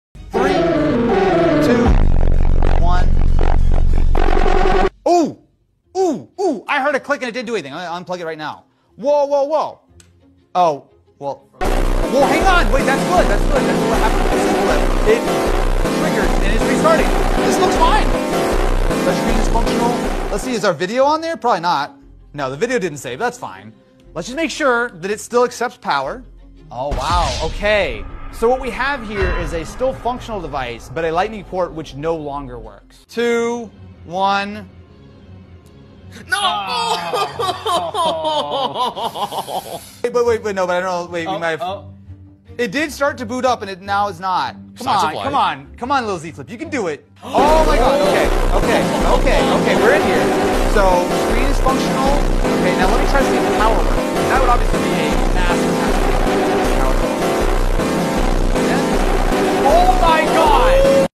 USB Killer Test: iPhone vs Samsung – Which One Survives?
iPhone vs Samsung: USB Killer Test – Shocking Results!
In this extreme durability experiment, we test a high-voltage USB Killer device on both an iPhone 16 Pro and a Samsung Galaxy S24 Ultra. Which phone can handle the surge? Which one fails instantly?

This real-time USB Killer test reveals the true internal protection of these flagship smartphones. Don’t miss the ending – it’s more shocking than the device itself!